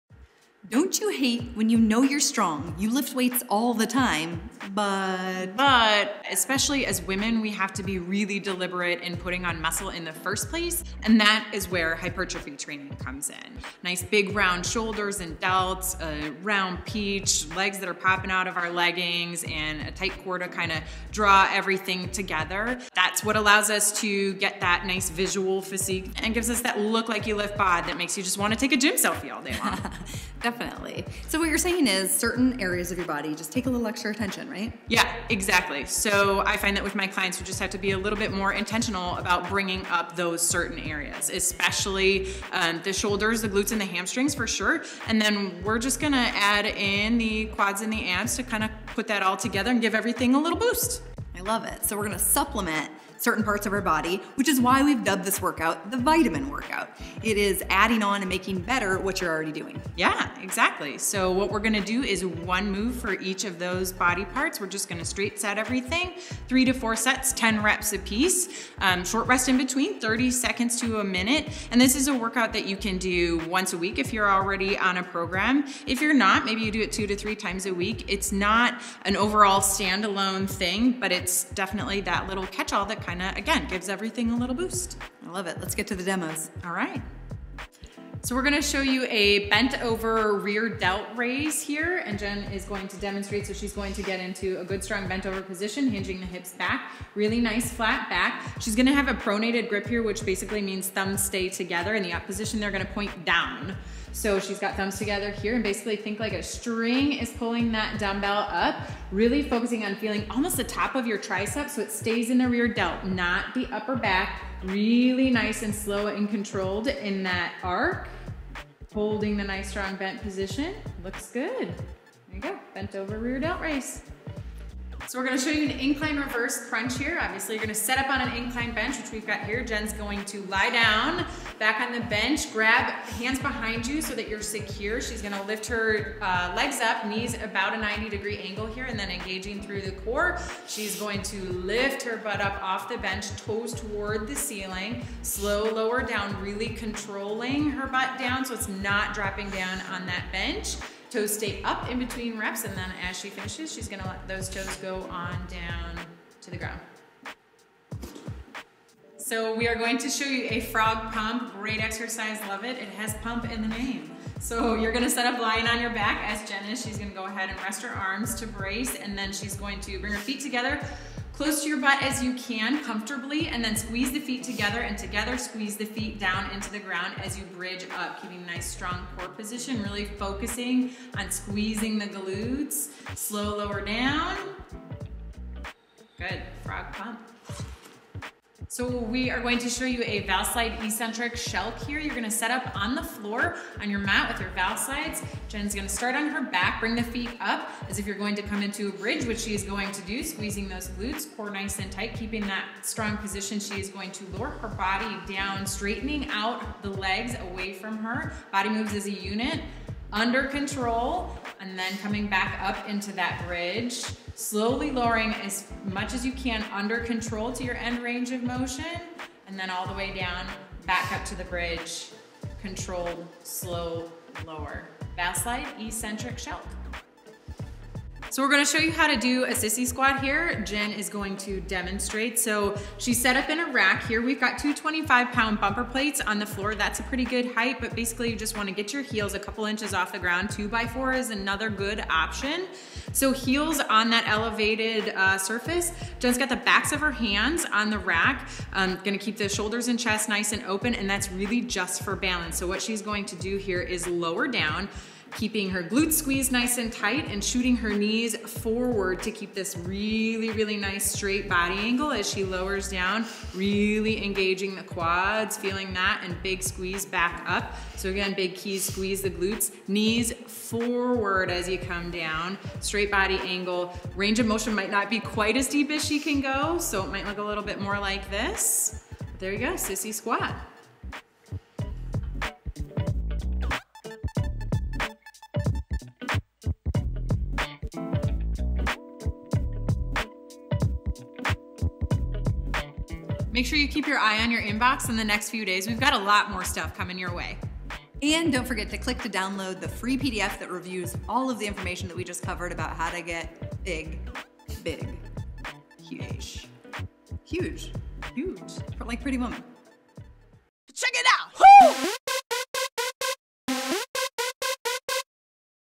How to Look Like You Lift
Amplify your results by paying extra attention to certain body parts. Supplement your rear delts, glutes, hamstrings, quads, and abs with trainer Kourtney Thomas’s “Vitamin Workout”! Perform three to four sets of 10 reps of each of the following exercises two to three times per week.

• Dumbbell Bent-Over Rear-Delt Raise
• Bodyweight Inclined Reverse Crunch
• Bodyweight Frog Pump
• Valslide Eccentric SHELC
• Bodyweight Sissy Squat

More details (plus a free downloadable PDF of the full workout!) at the blog post